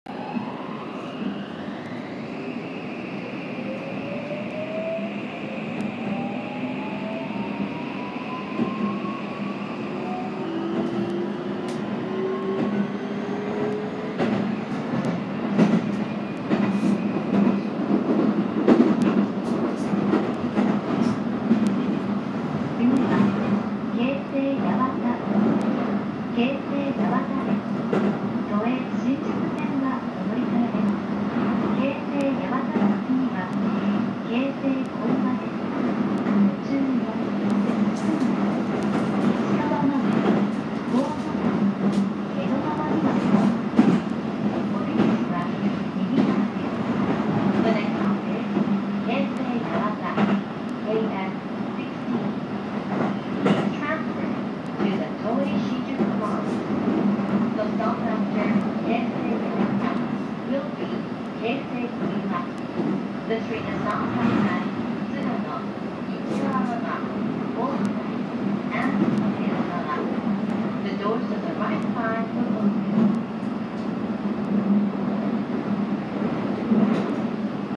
京成電鉄 快速羽田空港行き 東中山駅発車後車内自動放送
京成電鉄快速羽田空港行きの東中山駅発車後の車内自動放送です。